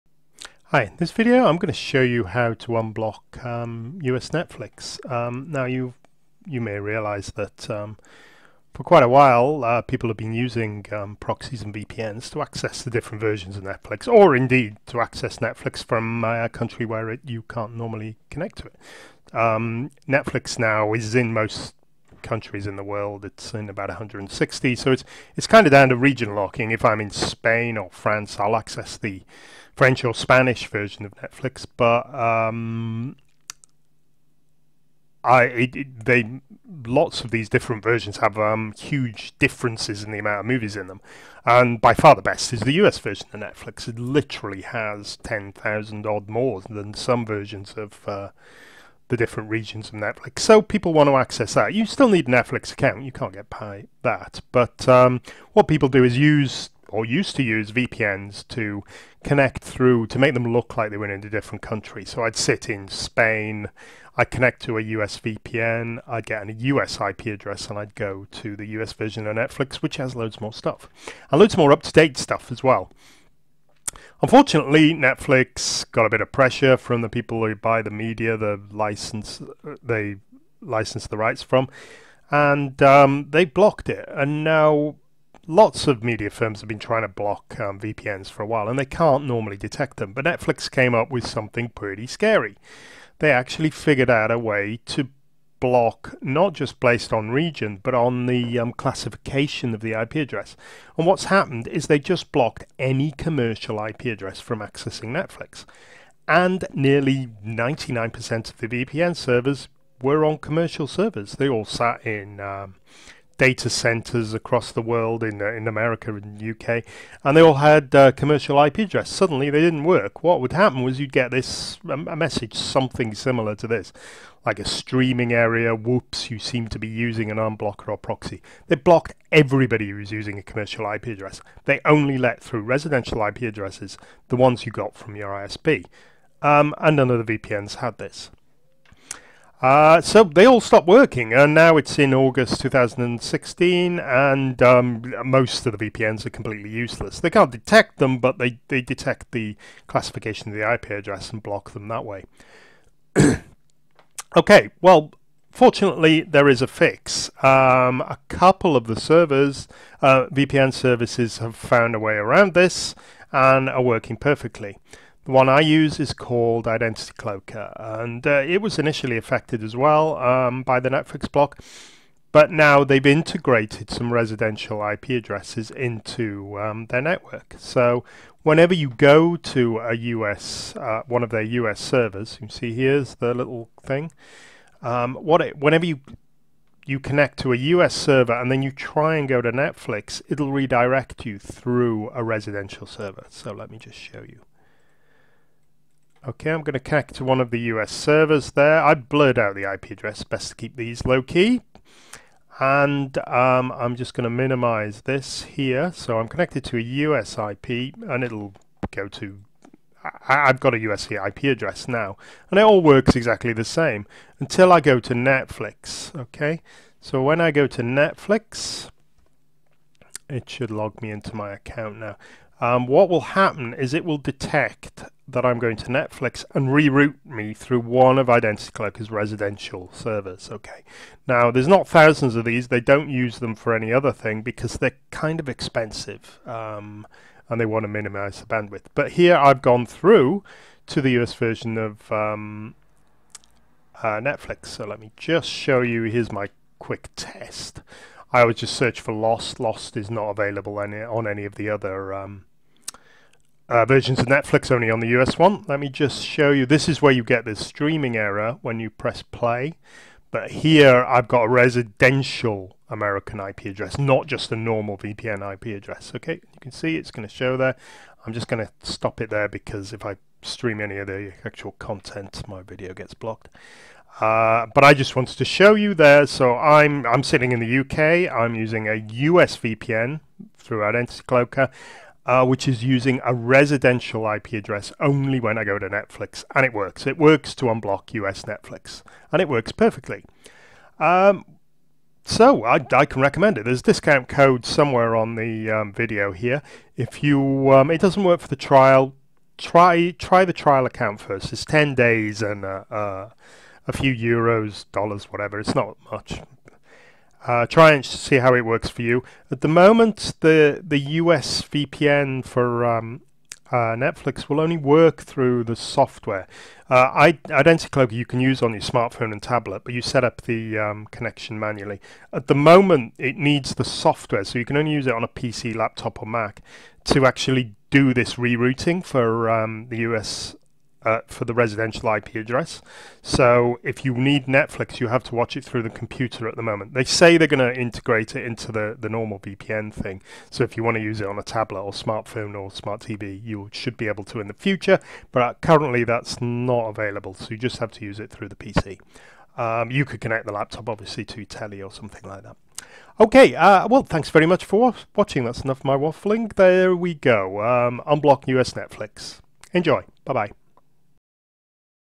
Unblock US Netflix in 2024
Important Update (2024) - Illustrated VPN no longer supports Netflix.

Netflix have implemented some technical blocks which have stopped the vast majority of VPN programs from working anymore.  However there is a solution and one VPN service continues to work with the US version of Netflix in 2016.

Watch this video and see a demonstration of how you can still unblock US netflix and access the thousands of films and movies which simply don't exist on other versions.